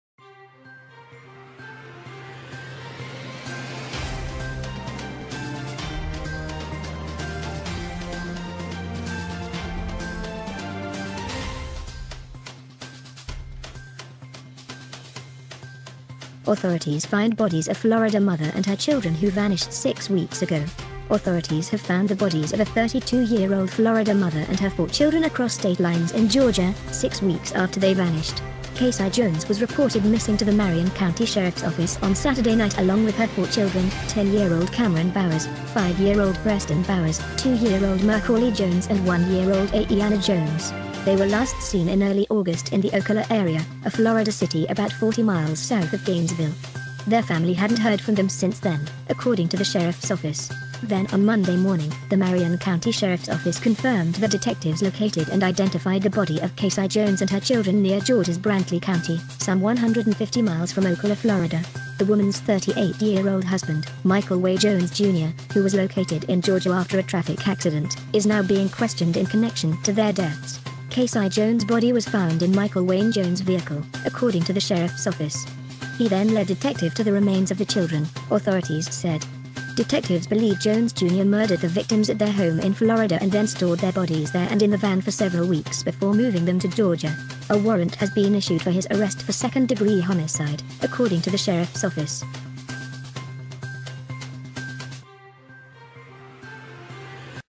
Authorities find bodies of Florida mother & children who vanished six weeks ago I DECENT NEWS I
Authorities have found the bodies of a 32-year-old Florida mother and her four children across state lines in Georgia, six weeks after they vanished.
Casei Jones was reported missing to the Marion County Sheriff's Office on Saturday night along with her four children, 10-year-old Cameron Bowers, 5-year-old Preston Bowers, 2-year-old Mercalli Jones and 1-year-old Aiyana Jones.